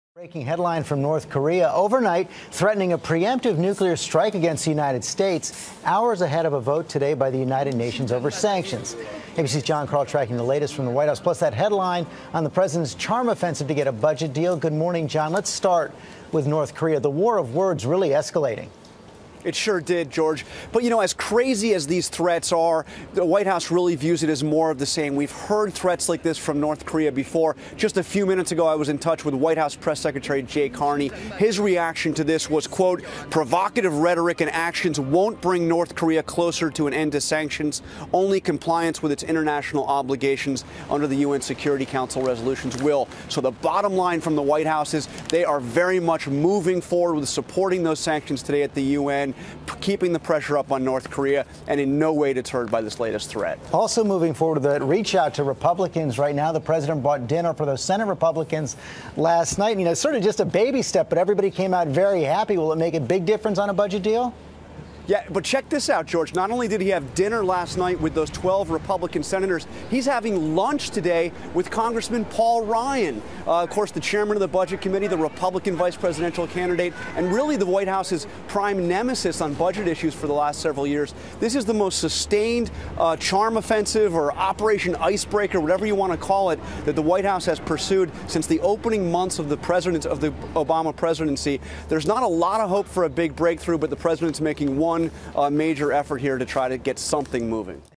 North Korea Leader Kim Jong Un Threatens a Pre-Emptive Nuclear Strike on U.S
North Korea vows nuclear action as the United Nations votes on sanctions.